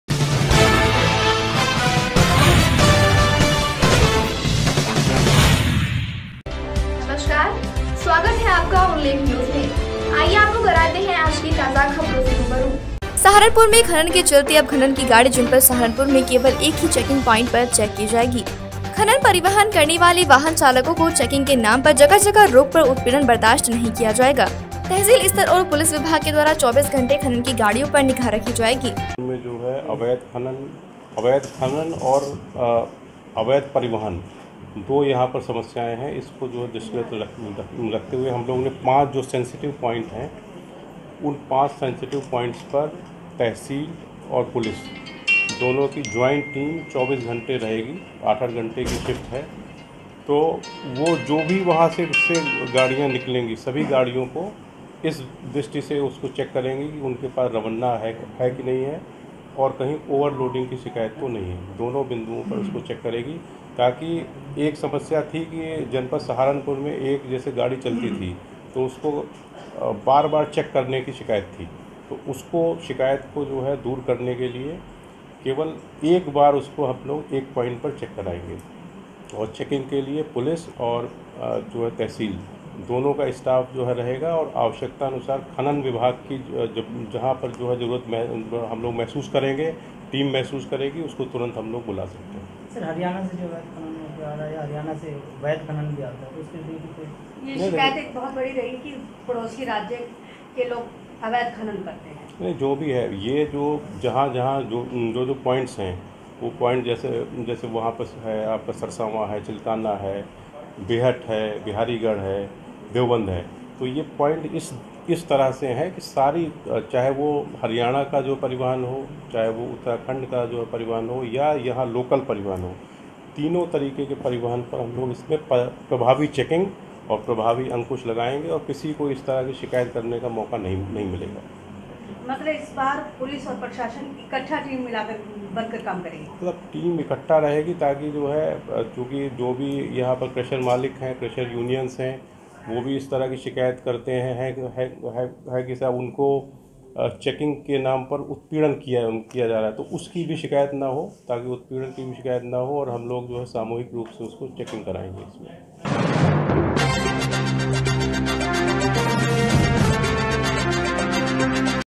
अवैध खनन को रोकने के लिए जिलाधिकारी अखिलेश सिंह ने कसा शिकंजा
सहारनपुर से निशांत गुप्ता की ब्यूरो रिपोर्ट